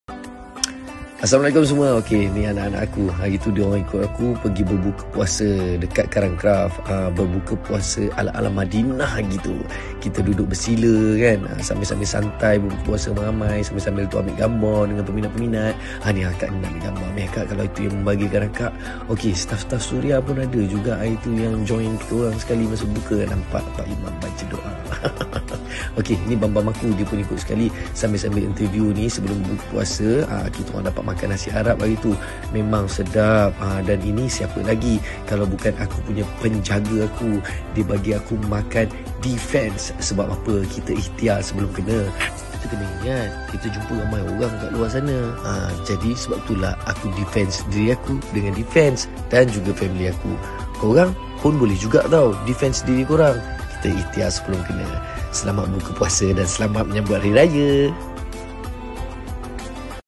Ajak Shiro | Eskayvie Diffenz | Eskayvie Malaysia
Posted @withregram • @ajakshiro Best juga sekali sekala berbuka ramai2 gini..Meriah naaa rasa😊 Sambil2 tu kena ingat sop kita sebab risau keadaan kat luar sana..Sebab tu saya pilih Diffenz keluarga saya, korang boleh dapatkan untuk Diffenz Junior jugak tau🤜🏽🤛🏽